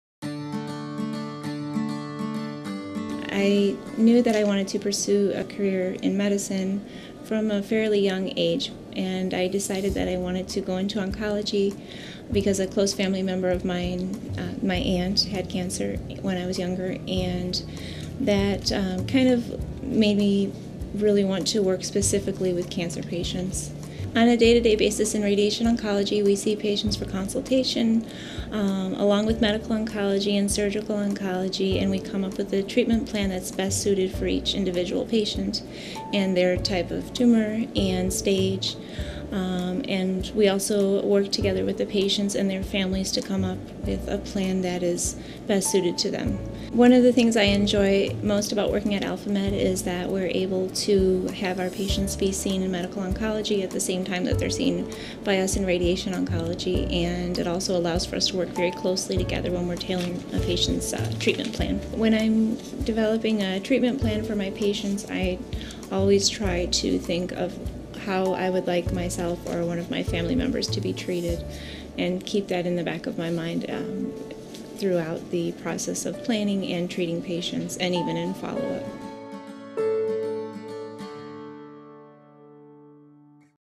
Meet Your Doctor: Dr. Angela Babbo
Meet Dr. Angela Babbo, Radiation Oncologist at Alpha Med Physicians Group.